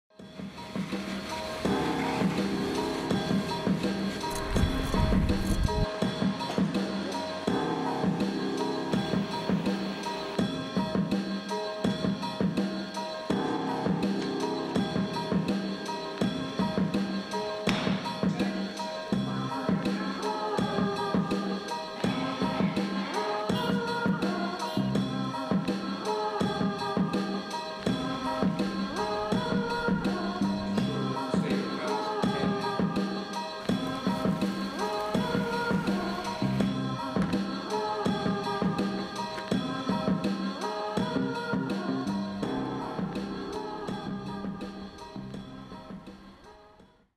Video Montage
This video montage is a brief look into some of the work I have done while at CBS Affliliate WABI-TV5.